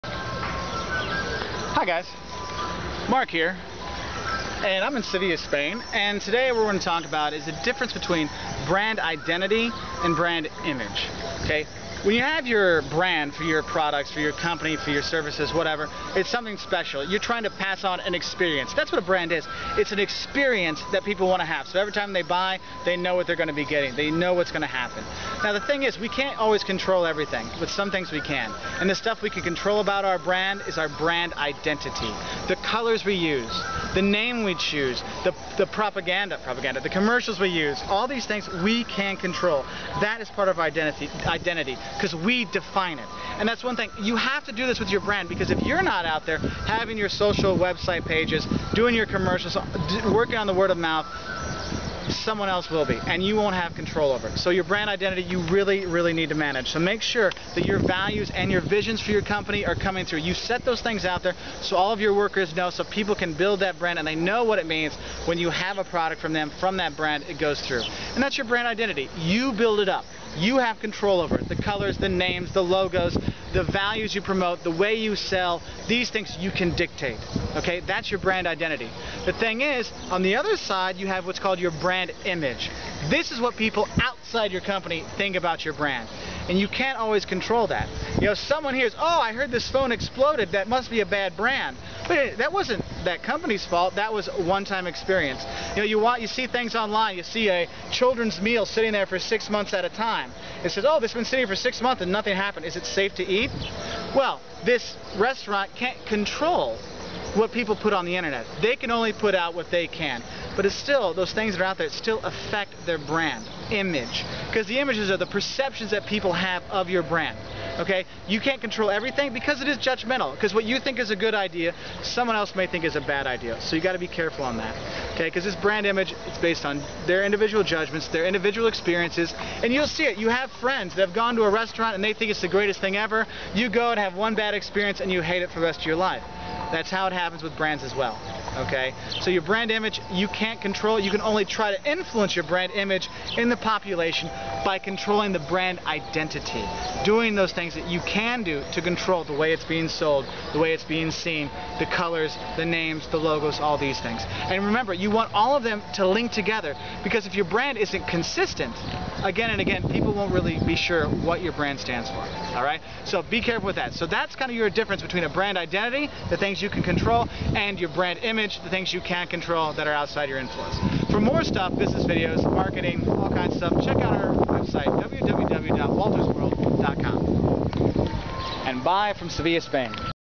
Branding 101: The Differences between Brand Identity & Brand Image
Companies sometimes feel like they can control everything about their brand. Sometimes they can, and that is called Brand Identity. As a company you can set out the colors, marketing scheme, tradenames everything to make your brand be what you want it to be. However getting customers to have that same brand identity is difficult. Because Brand Image is what people/customers perceive your brand to be and this can come about by their own experiences that you cannot control.

Filmed in Seville, Spain

marketing and branding explained